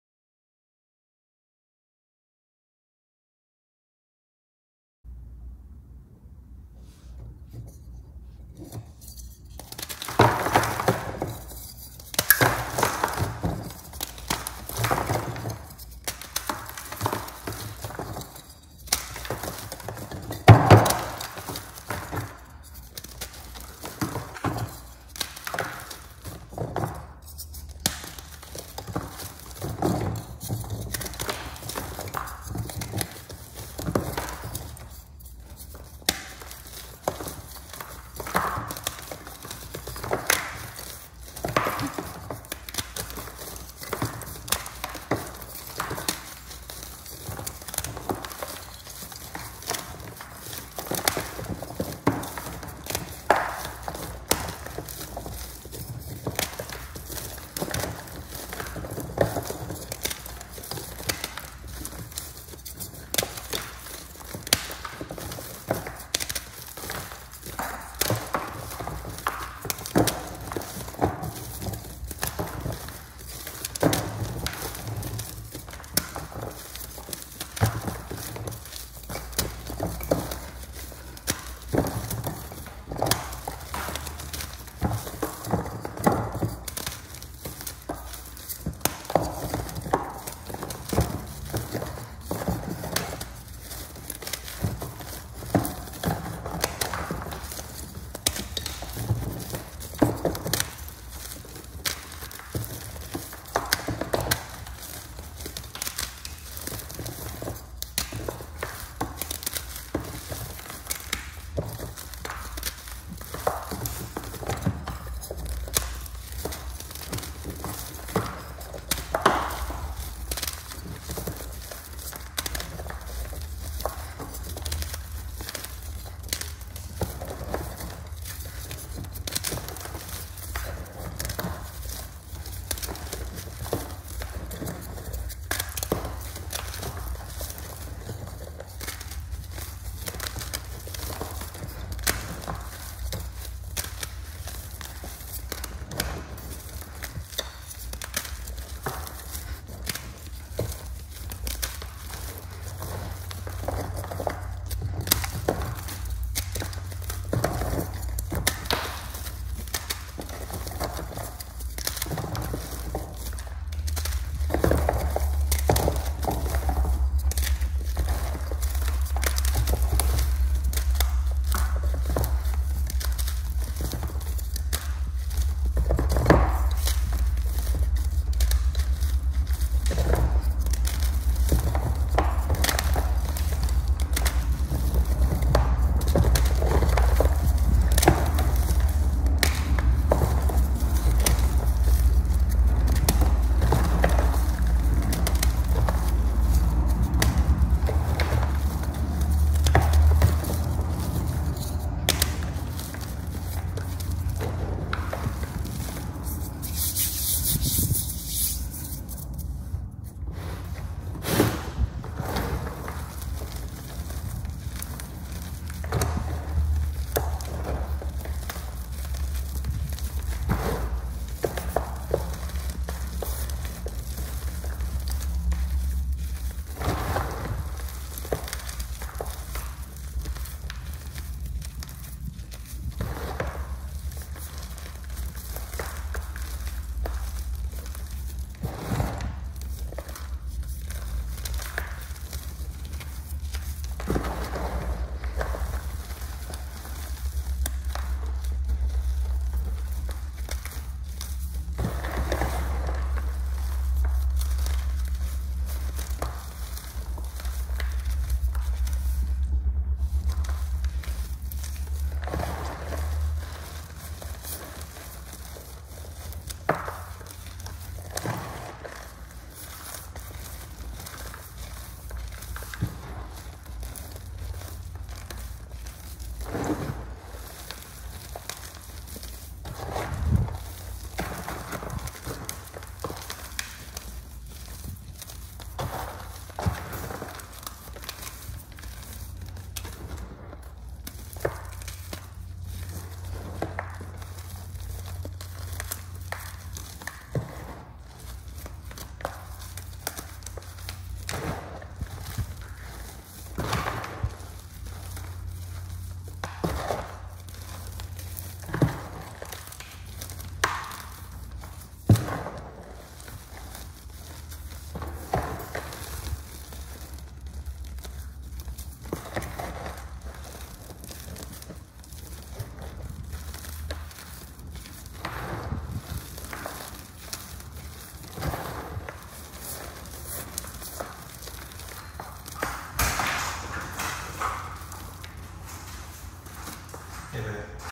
100 Gym Chalk Speed Crush | inspireseries | XXX
This is the first post of my inspireseries. I wanted to pay tribute to five gym chalk ASMRist who has inspired me😃. Today's video was inspired by @CrunchyASMR84 the link to the original video is below

Now when it comes to her videos, the iconic video that got me was her OG golden disks😊. They sounded so crunchy and delicious! Unfortunately, I don't have molds the same size as hers so I decided to pay tribute to a more recent video of hers. We all know she goes in on the mass colored reforms (they be poppin)! I just wanted to take today & pay tribute to all of her hard work and crunchy reforms! I was so excited to do my first ever large reform crush! Hope you all enjoy the video.

Do you have any Gym Chalk ASMRist who inspire you?